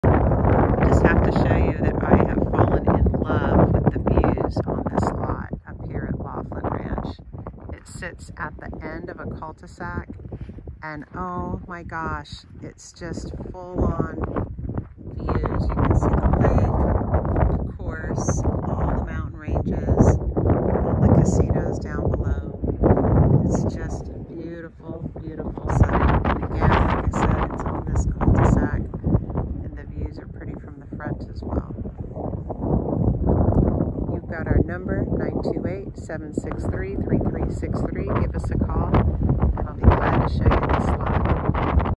Troon Golf Course Lot For Sale with Mountain Views! (Vacant Land For Sale - Bullhead City Arizona)
Make 3298 Schooner Cove in Laughlin Ranch at the Vistas you new homesite.  End of cul-de-sac with outstanding 180 degree views to the south and south east.  Troon golf course views and majestic mountain views along with private location are just what you have been looking for.  I love this lot.  We have others to choose from too.